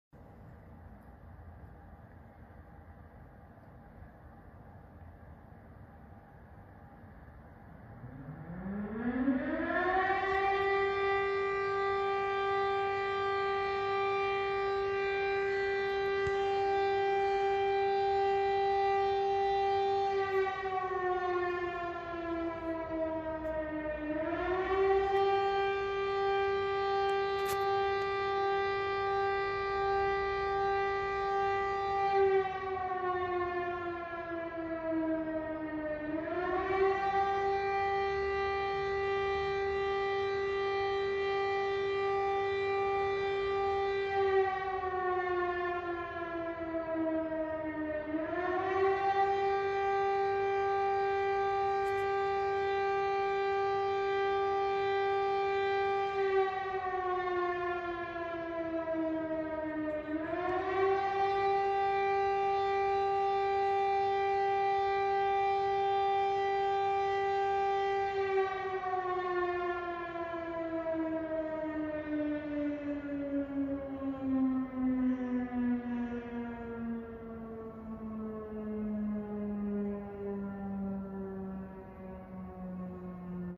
3 sirène alerte PTR Clermont Ferrand PAU et Vesoul pour Roblox
0:00 sirène alerte PTR mairie de Clermont Ferrand les barres octogonales sont sous les pavillons 4 fil qui les maintiennent

0:06 ancienne sirène alerte PTR remplacé par un Pakita LC mairie de PAU 4 câbles qui maintiennent les barres octogonales au dessus des pavillons

0:11 sirène alerte PTR mairie de Vesoul 4 fil qui maintiennent les barres octogonales sur les pavillons de la sirène alerte